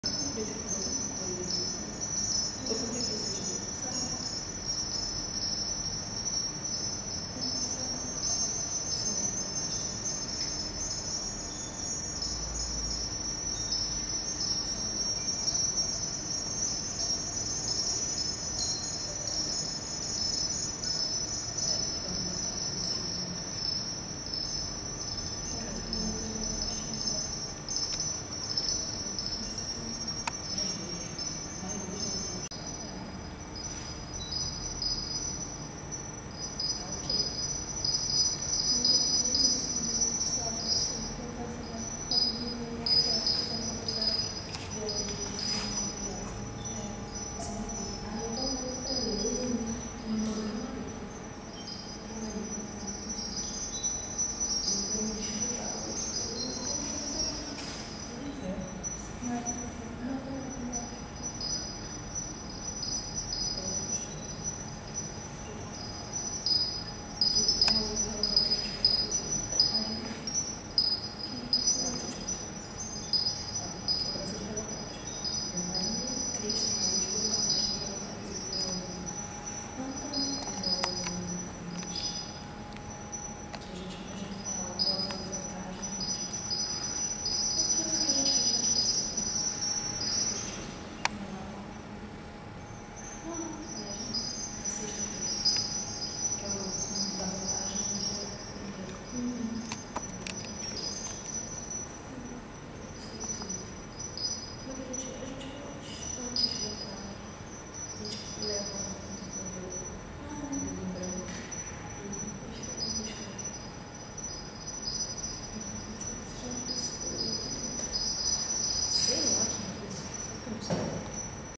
Image Recoder Arte Forum in Rio
Richard Colson's Image Recoder demonstrated at Arte Forum June 2011 Federal University of Rio de Janeiro. With thanks to Marymilia Fatah and Jean Martin (sound)